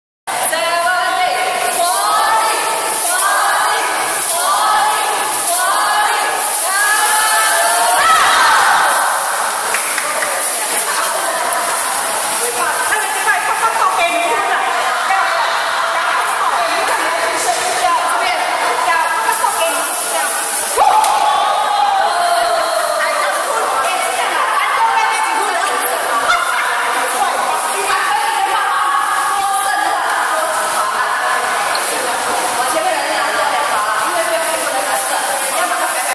LaLa Team by Blk.351 Auntie
Taman Jurong Zone D Line Dance. Blk.351A Auntie La La Team Funny Practise!